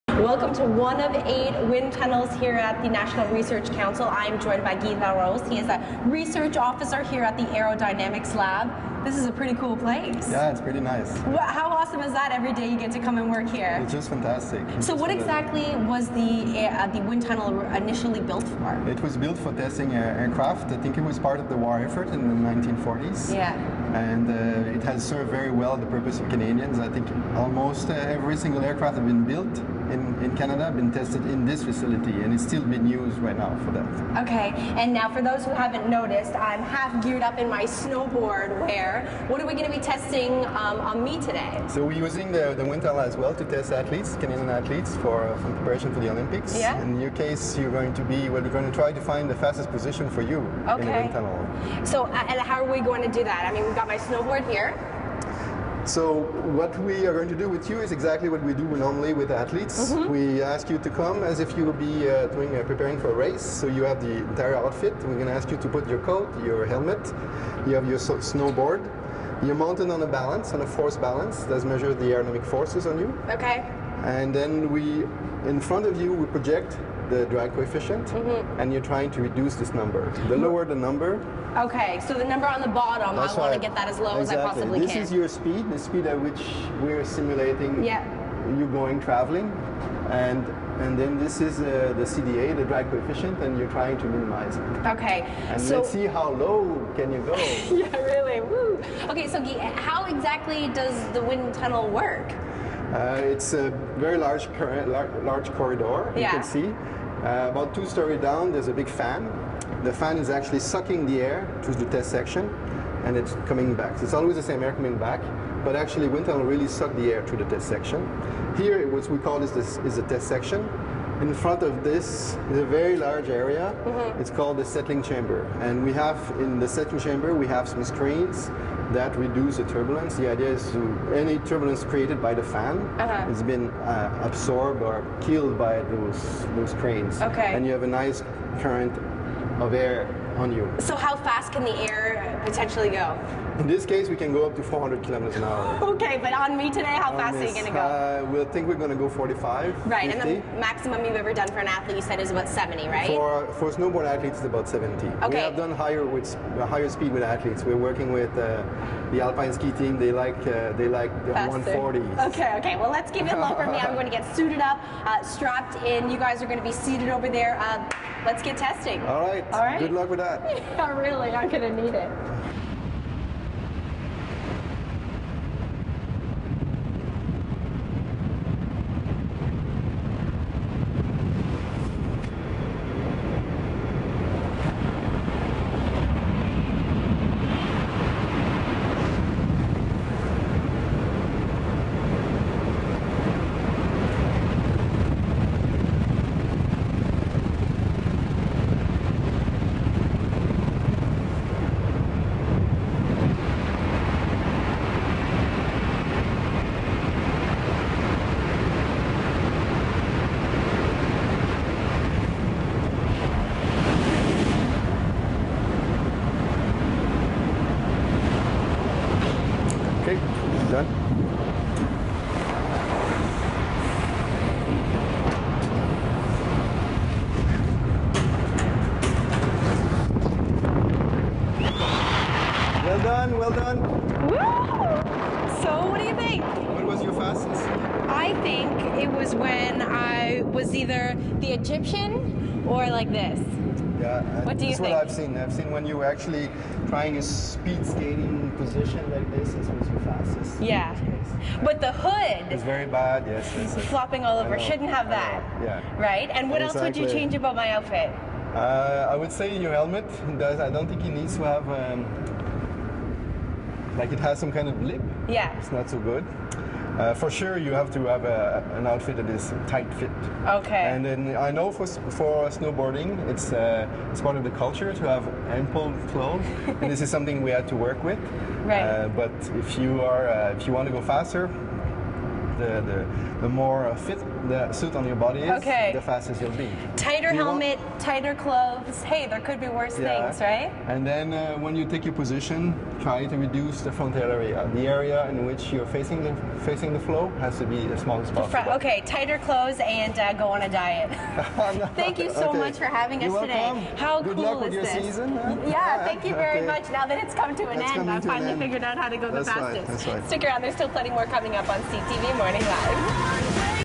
NRC - Wind Tunnel
Anyone else feel a draft? CTV Morning Live's Sarah Freemark gets involved in some aerodynamics as she visits the Wind Tunnel at the NRC.